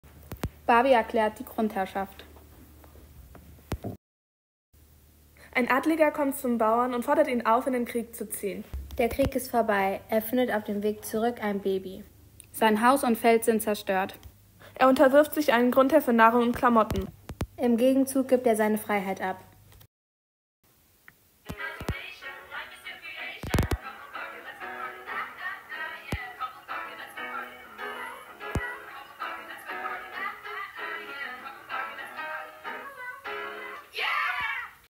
Legetechnik in der 8a (Film 2): Ein Workshop zum Thema "Herrschaft im Mittelalter"
Am Dienstag dem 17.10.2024 fand in unserer Klasse ein Workshop zum Entwerfen von Erklärvideos
zum Thema "Herrschaft im Mittelalter" statt. Vorab hatten wir die Themen mit unserem Geschichtslehrer (Herr Bramann) vereinbart und erste Skizzen wurden im Geschichtsunterricht angefertigt. Somit konnten wir, nachdem uns alles von dem Medienpädagogen Urs Daun erklärt worden war, direkt mit dem Aufzeichnen beginnen. In Gruppen hatten wir bis zu sechsten Stunde Zeit, zu filmen und kreativ zu werden. Als in der siebsten Stunde alle fertig waren, haben wir uns gemeinsam unsere Werke angeschaut. Von klassischen Erklärvideos bishin zu "Barbie erklärt die Grundherrschaft" war alles dabei! Den Workshop würden wir anderen Klassen weiterempfehlen.

Text: Viola Bartoschek, Carina Meinel, Lisa Zasada